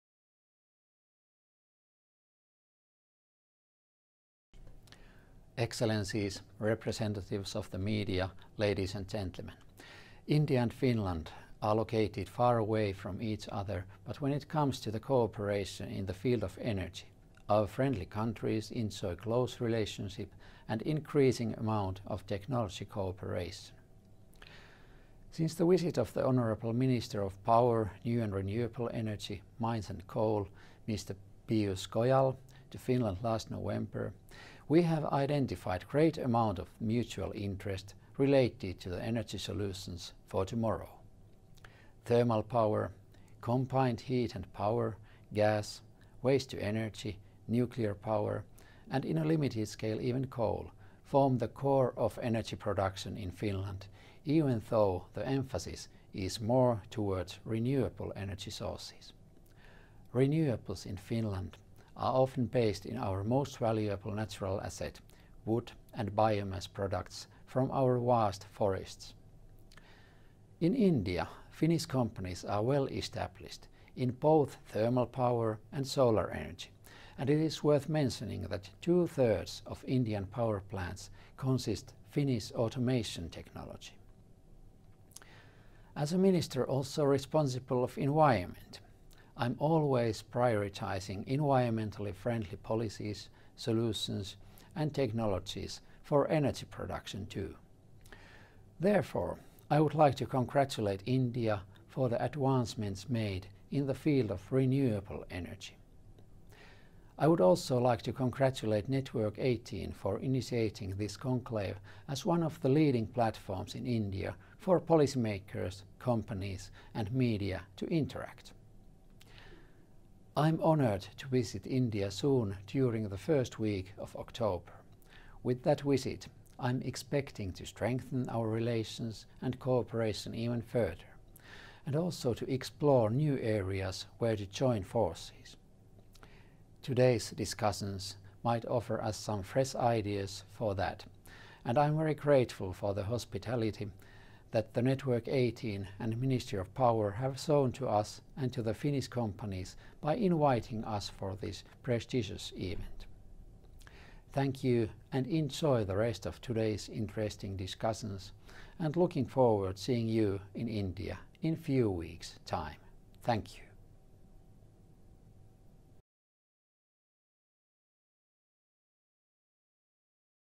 Opening words by Minister Tiilikainen in India Power Conclave 2017
Opening words by Minister Tiilikainen in India Power Conclave, 22th of September 2017 in New Delhi.